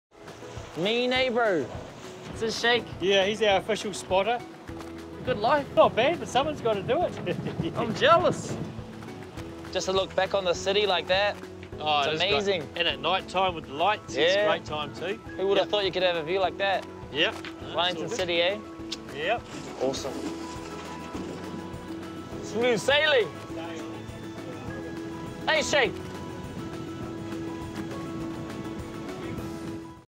Wild Idea: Set Sail on Wellington Harbour
There are hundreds of ways to have a wild weekend in Wellington. In this mini-sode Astley Nathan sets sail on the Wellington Harbour with First Mate, Shake.

Plan your own wild weekend at WellingtonNZ.com

Location: Megisti Sailing Charters, Wellington Harbour.